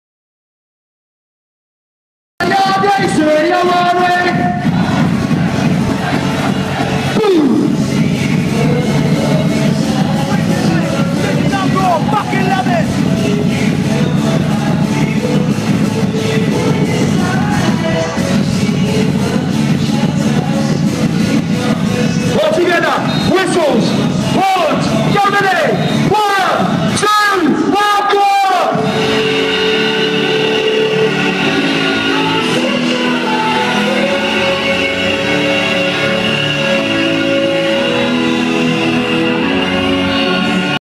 Uproar We Bring the Noise with MC Storm and lasers
Uproar We Bring the Noise on Saturday February 2nd 2013 with MC Storm and lasers recorded and edited by ravinpix